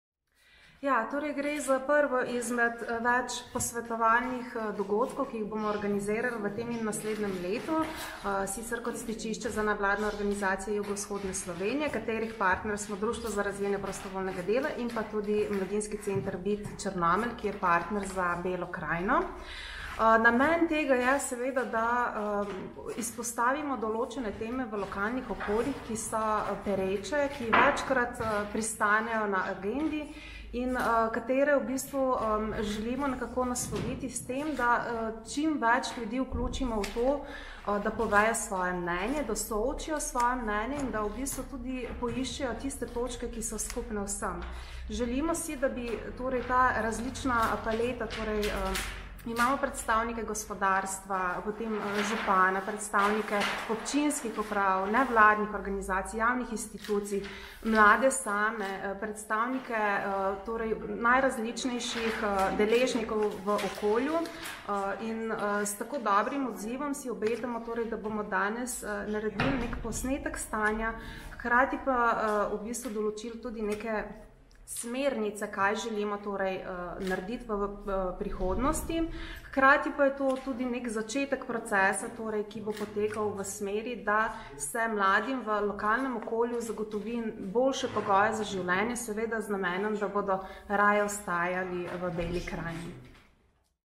Maja Žunic Fabjančič NVO
Zk mladi zapuščajo Belo krajino? Črnomelj, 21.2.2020